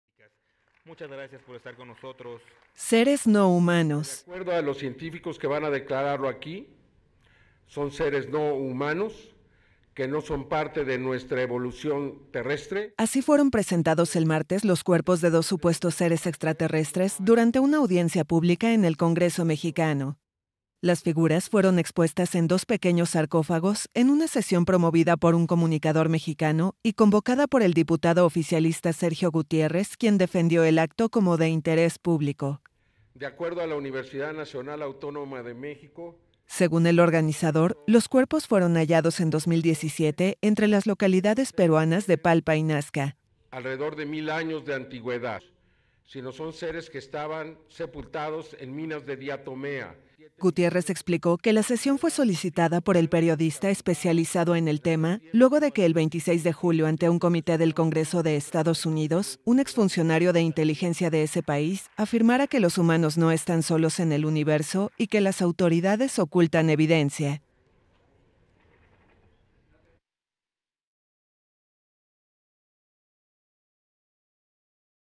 Exhiben cuerpos de supuestos extraterrestres en el Congreso mexicano | AFP
Los supuestos cuerpos de dos seres extraterrestres fueron exhibidos durante una audiencia pública en el Congreso mexicano, que por primera vez abordó el tema de manera oficial en el país.